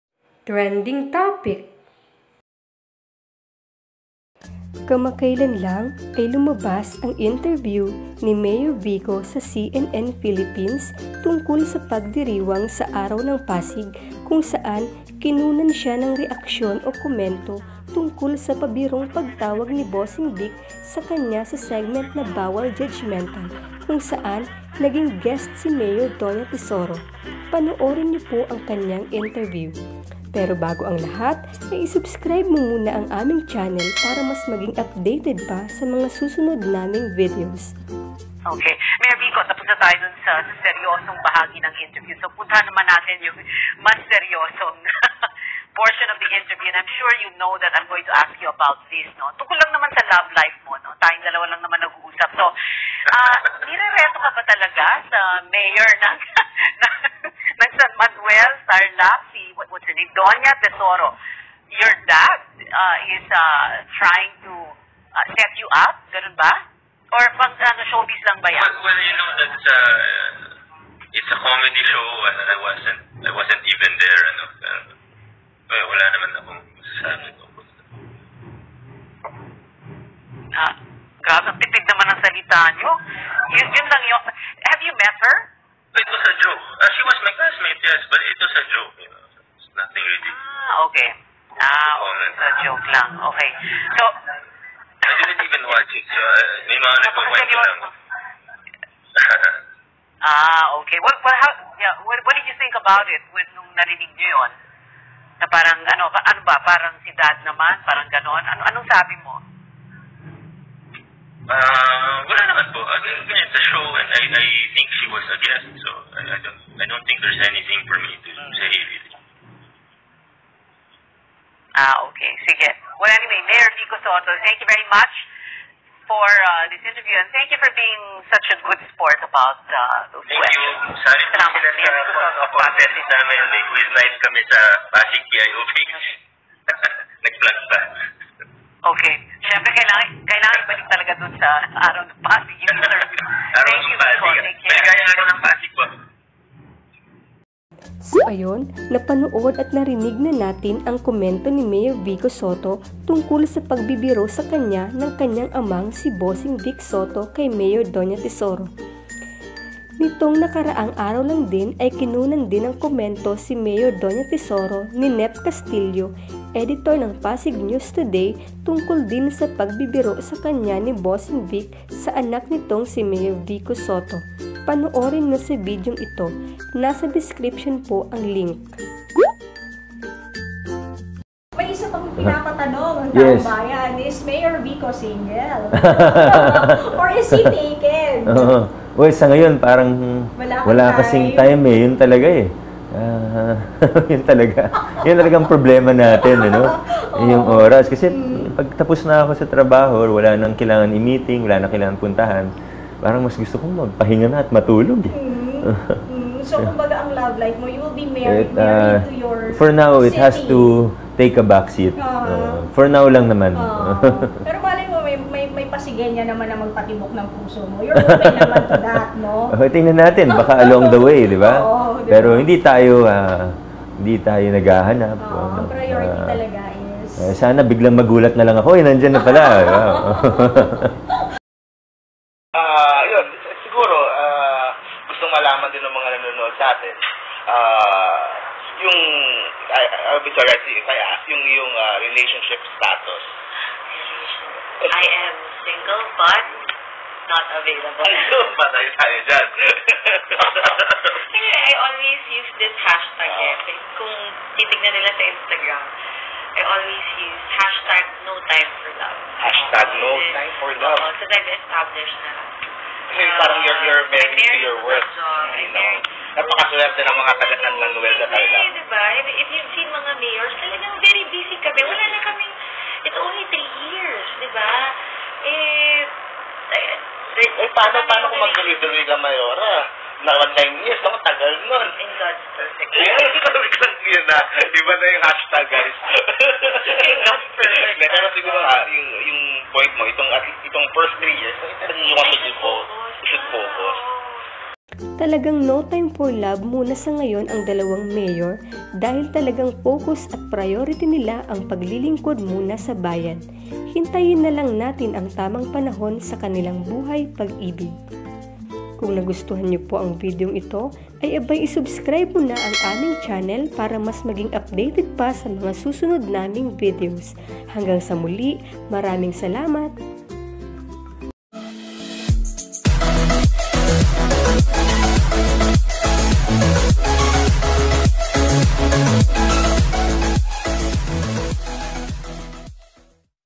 ANG RASON KUNG BAKIT NIRETO NI BOSSING VIC SI VICO SOTTO KAY DONYA TESORO
Mapapanood sa video na ito ang interview ni Mayor Vico Sotto sa CNN Philippines nitong nakaraang Araw ng Pasig. Naitanong sa kanya kung ano ang kanyang komento sa pagbibiro ng kanyang amang si Bossing Vic na kunwaring tinawagan siya habang nandoon sa studio si Mayor Donya Tesoro dahil guest si Mayora sa segment na Bawal Judgmental.
Panoorin niyo hanggang huli ang video at huwag niyong kakalimutang magsubscribe.

Maraming Salamat!

Video clip credit to:
Models of Manila
News 5
CNN Philippines

 Fan Made Video only ❤️